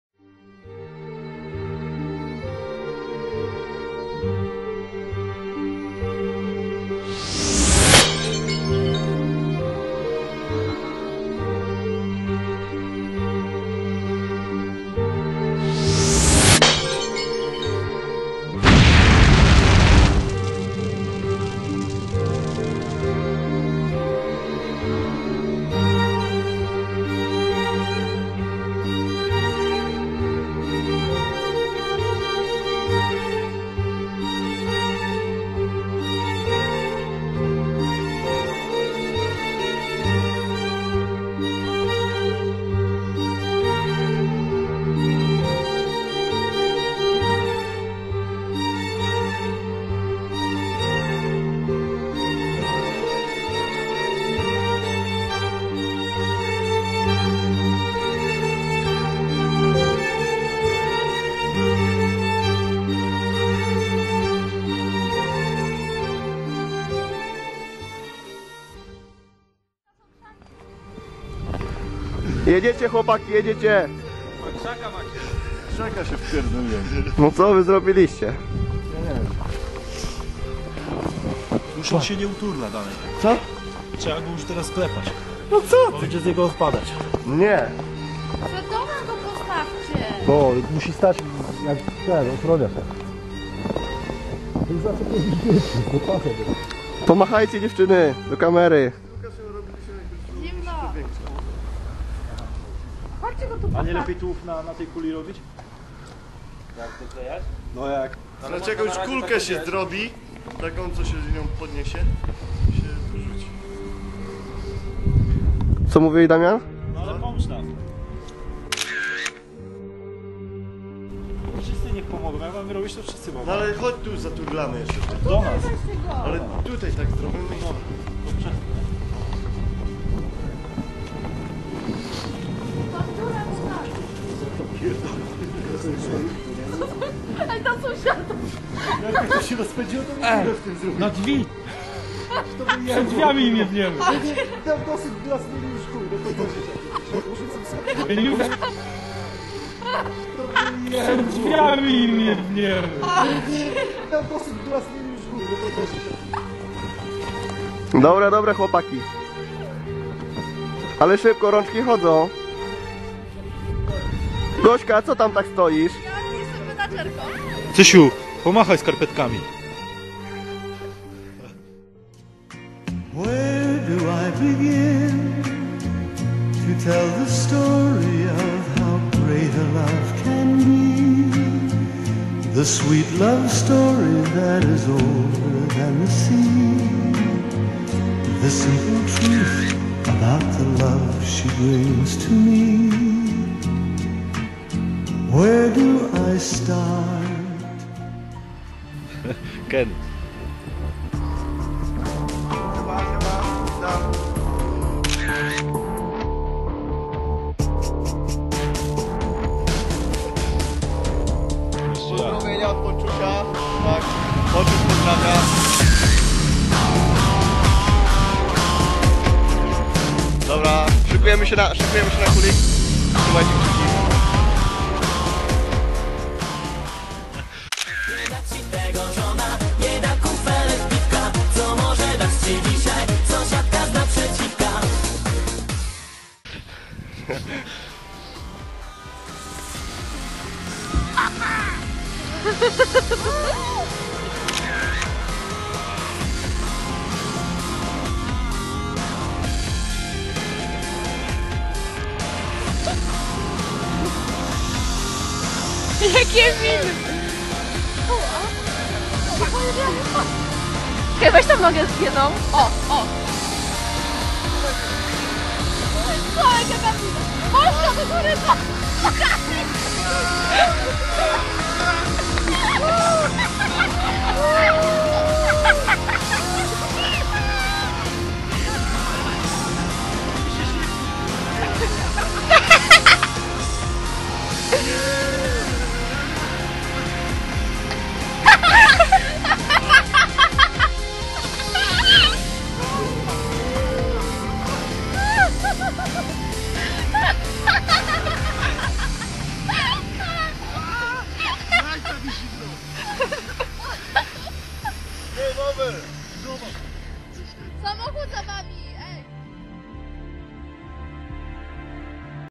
Guildford kulig-Luty 2009 Part 1
To moj pierwszy film zmontowany w Sony Vegas Movie Studio, komentarze mile widziane :) Razem ze znajomymi zrobilismy kulig po obfitych opadach sniegu w Anglii.Milego ogladania =)